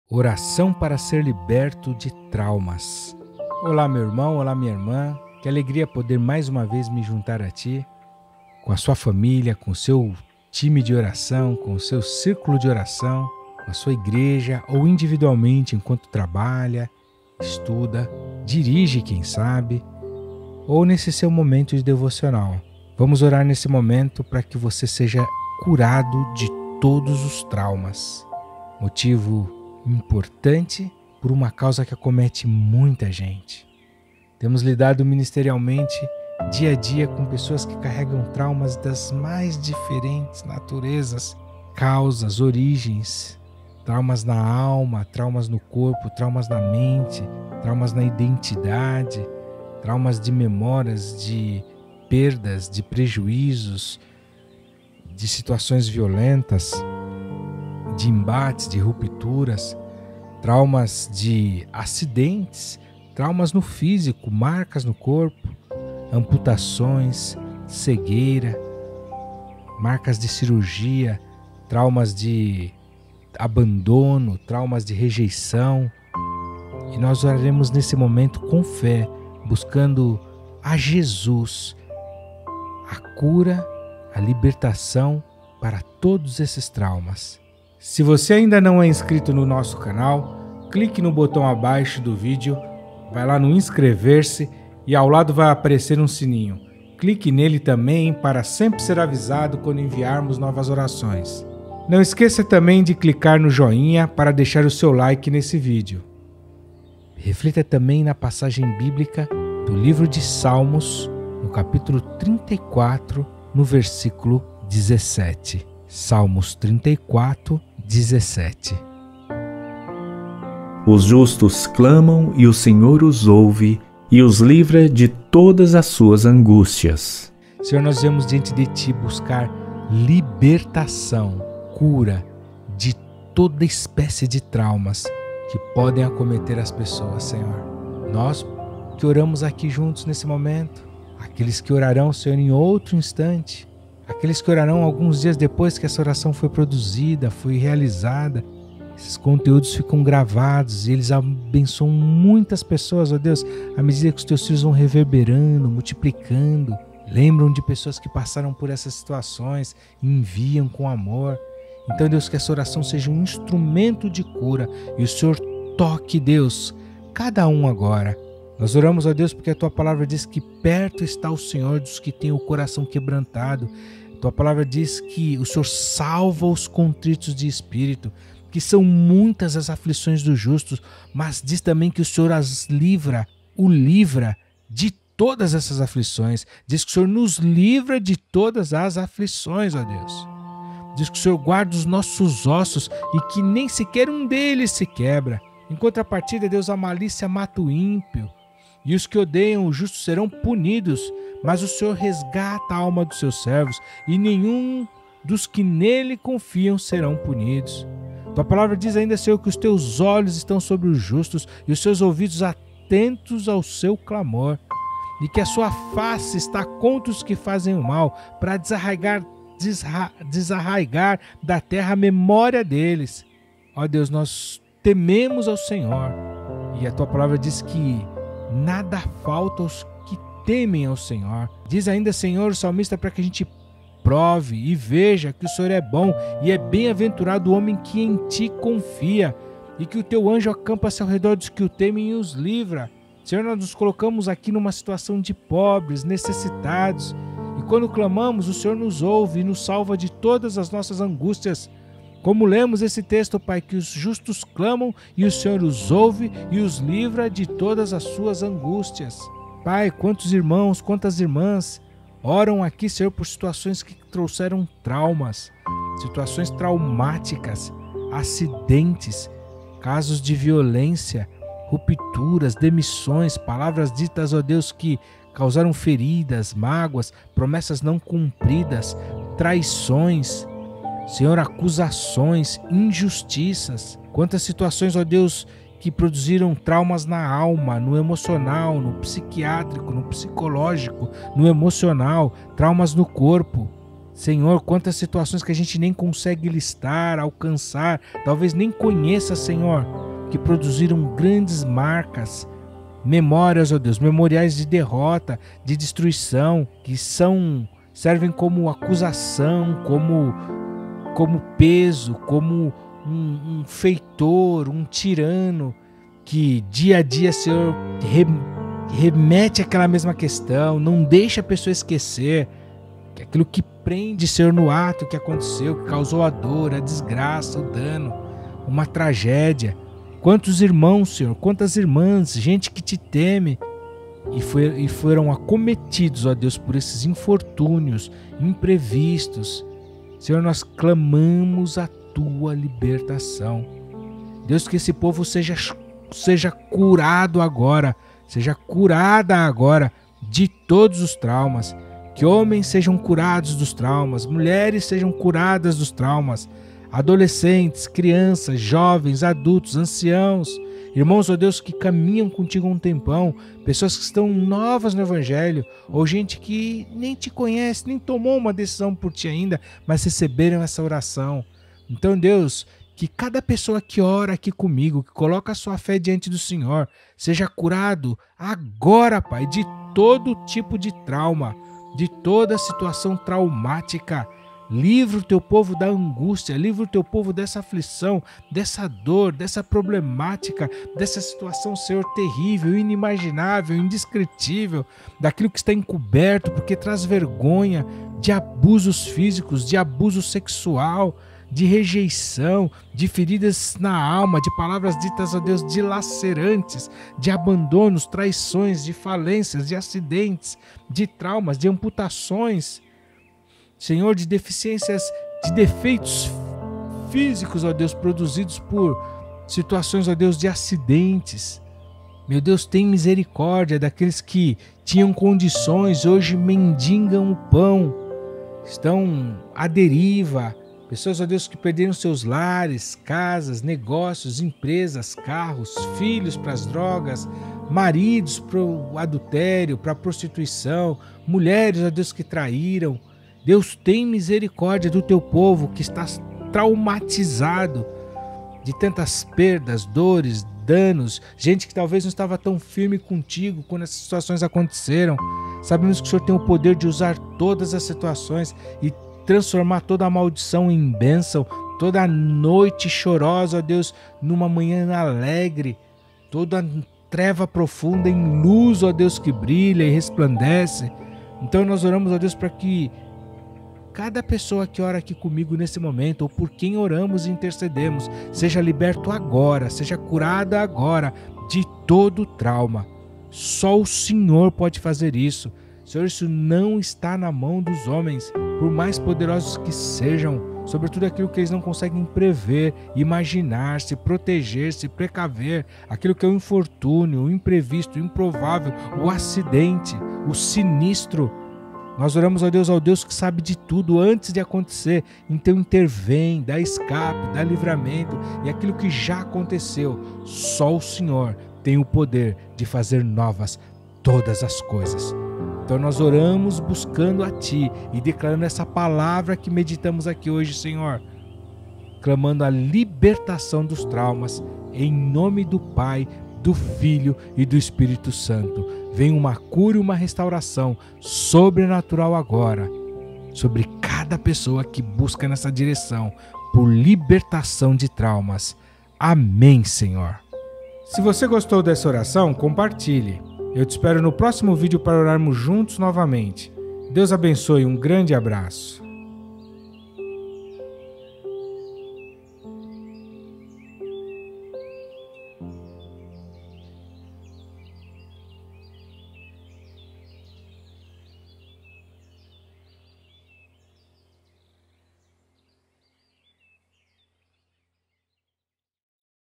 ORAÇÃO DA MANHÃ | PARA SER LIBERTO DOS TRAUMAS 🚫 - Bíblia JFA Offline
Quais são os seus traumas? Deixe nos comentários! E se você precisa de ajuda ou atendimento pessoal, contate nossa equipe através do WhatsApp

0:00:00 - Oração para ser liberto dos traumas